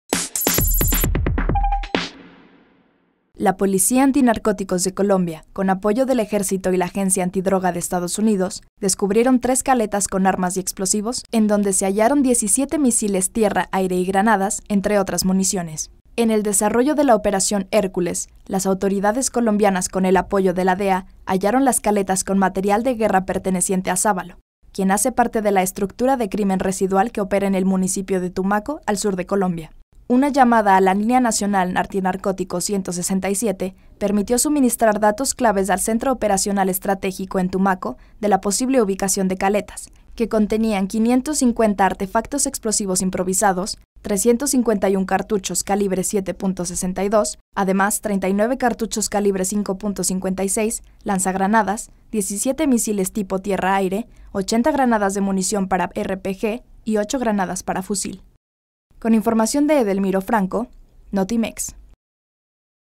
Hallan 17 misiles tierra aire en sur de Colombia
La policía Antinarcóticos de Colombia, con apoyo del Ejército y la Agencia Antidroga de Estados Unidos, descubrieron tres caletas con armas y explosivos